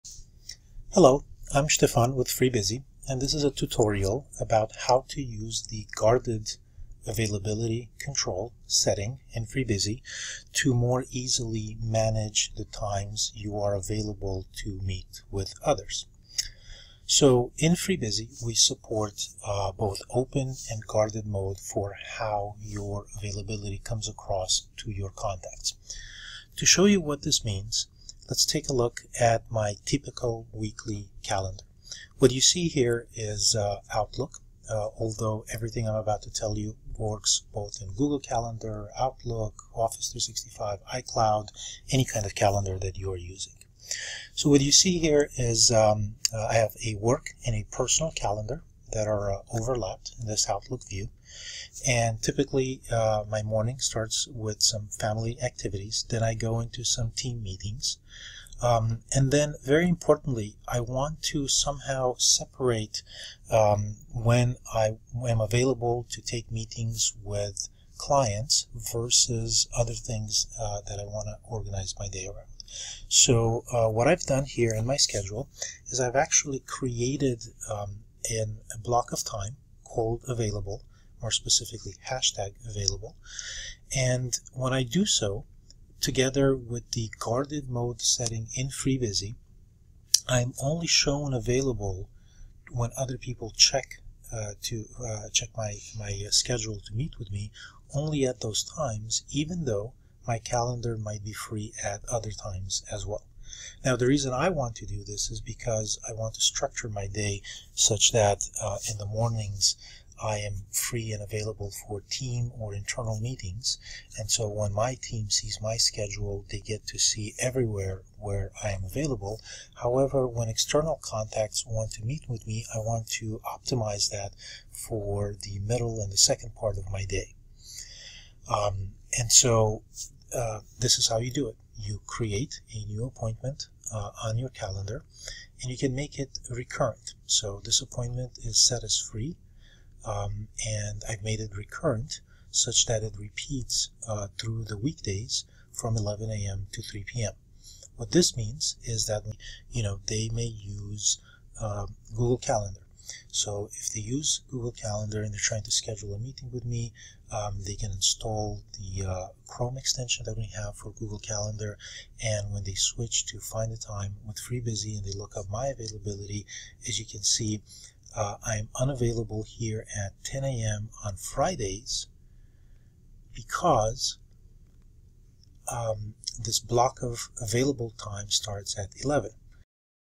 FreeBusy Guarded Mode Tutorial
This is a tutorial on how to easily manage the times you are available to meet with others, by controlling your availability in FreeBusy with the Guarded Mode setting.

In FreeBusy we support Open and Guarded mode for how your availability is shown to other people when they want to meet with you. Open means you are open to take meetings so if there’s nothing on your calendar at a particular time, you appear available. Guarded means you will appear unavailable during the day. To show yourself available at specific times, create events marked "Available" or "Free" on your calendar.

To show you what this means let's take a look at my typical weekly calendar. What you see here is Outlook, although everything I'm about to tell you with any kind of calendar you are using. So what you see are a work and personal calendar overlapped in Outlook. Typically my mornings start with some family tasks then I go with some team meeting and then, very importantly, I want to somehow separate when I'm available to take meetings with clients vs. other things that I was to focus on. So what I've done here, on my schdule, is I actually created a block of time titled "available". This works in concert with the Guarded Mode setting in FreeBusy I'm only shown available when other people check my schedule to meet with me only at those times even though my calendar might be free other times as well.

Now, the reason I want to do this is because I want to structure my day such that in the morning I'm free and available to for team or internal meetings. So when my team check my schedule they see everywhere when I'm available, however when external people meet with me I want to optimize that for middle and the second part of my day. And so, this is how you do it. You create an appointment on your calendar and you can make it recurrent. The appointment is set to show as "Free" and I made it recurrent such that it repeats on weekdays from 11am to 3pm. This means that when somebody checks my availability, say they may use Google Calendar. They can install the Chrome extension we have for google Calendar and when they check the tab "Find a time with FreeBusy" the see that I'm not available Fridays at 10am. And that because the "available" time block starts at 11am.